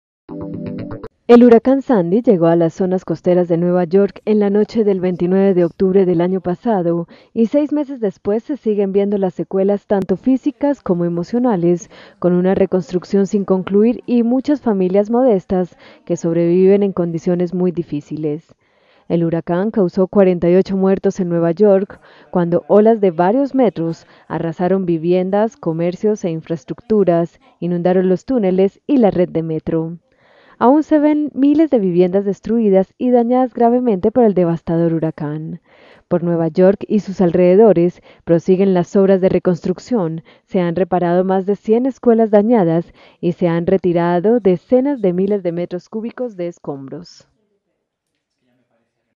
Después de seis meses las secuelas de "Sandy" aún se ven
Seis meses después de que el huracán "Sandy" arrasara buena parte de las zonas costeras de la ciudad, Nueva York no ha cerrado aún las heridas físicas y emocionales, con una reconstrucción sin concluir y muchas familias modestas que sobreviven en condiciones muy difíciles. (Video EFE)